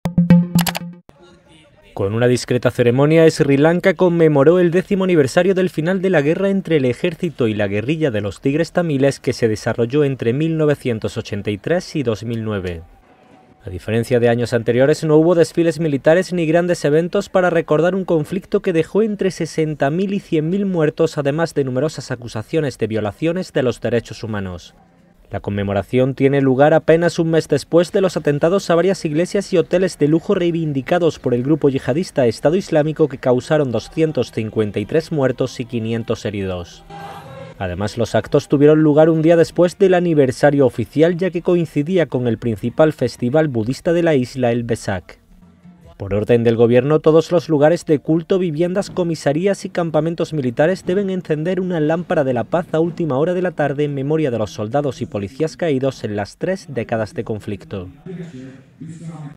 Sri Lanka conmemora diez años del fin de la guerra
Colombo (Sri Lanka), 19 may (EFE/EPA).-(Imagen: Pushpa Kumara) Las autoridades de Sri Lanka conmemoraron hoy discretamente el décimo aniversario del final de la guerra entre el Ejército y la guerrilla de los Tigres Tamiles (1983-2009), mientras todavía están latentes los miedos tras los ataques terroristas que el mes pasado causaron 253 muertos y 500 heridos.